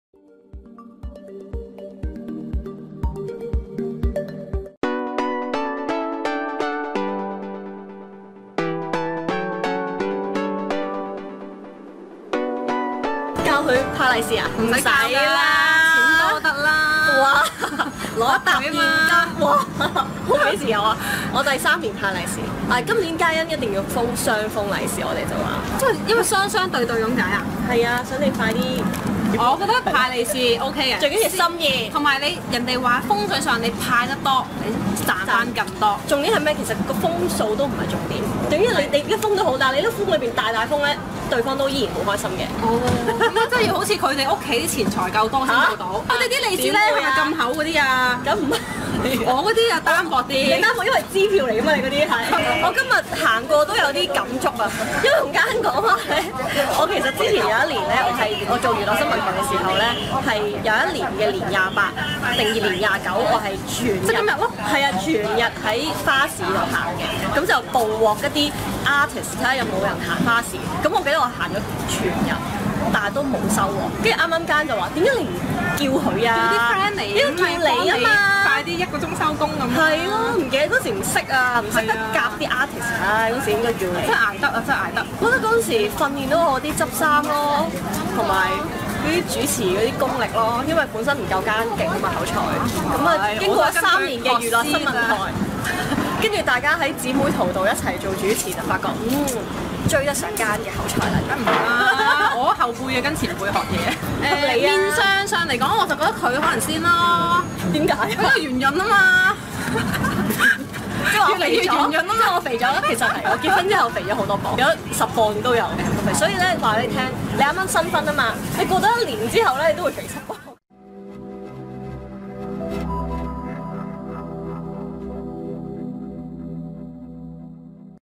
蔡嘉欣自認同老公感情變淡 陳詩欣踢爆：就嚟分房瞓
藝人蔡嘉欣和陳詩欣向來感情要好，日前2人相約買年花，為屋企加添過年氣氛。